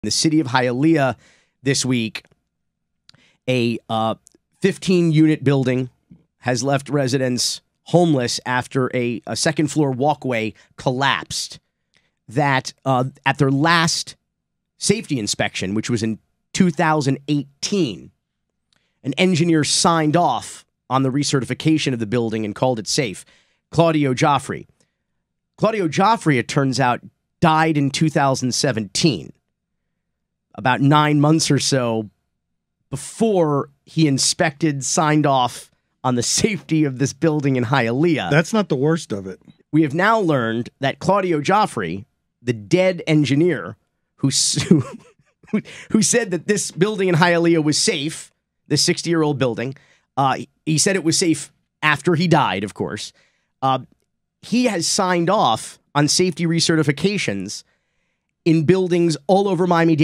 The Haunting of Hialeah
Deceased inspector Claudio Jofre is still certifying buildings in Miami-Dade County from the grave.